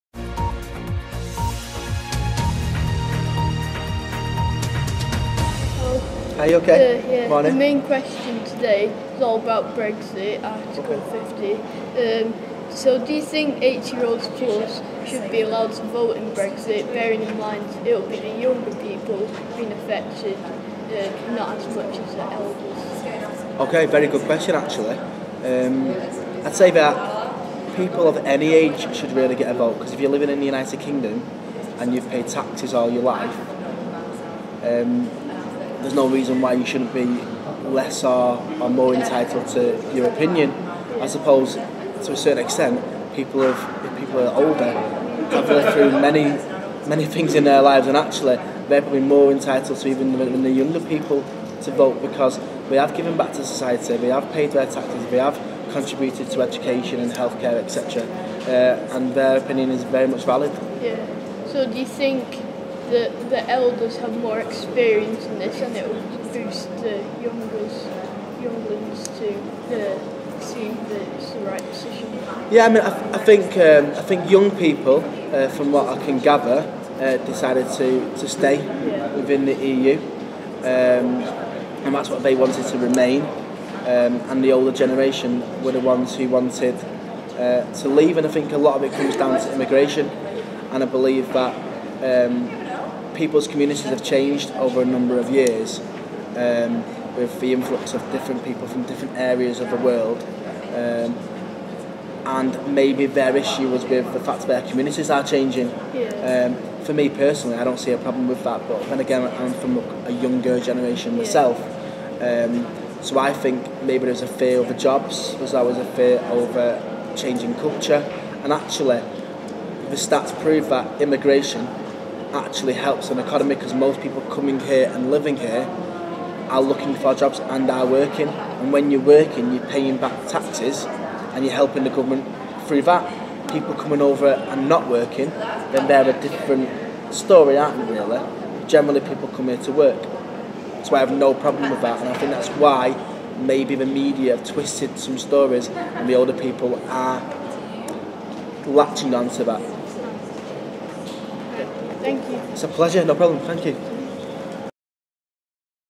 Brexit - New Charter Academy
Mr Jacques talks about the impact of leaving the european union.